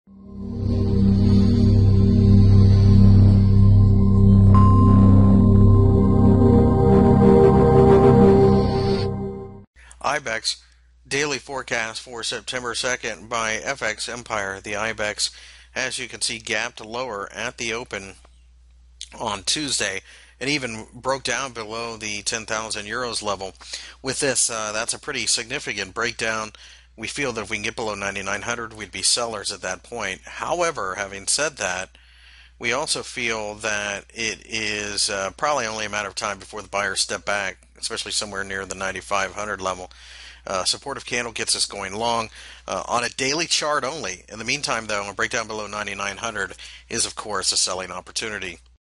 IBEX 35 Technical Analysis for September 2 2015 by FXEmpire.com
- September 2 2015 index daily technical analysis for the IBEX. Find more information about Forex and Commodities News Technical Analysis Fundamental Analysis and Brokers on FX Empire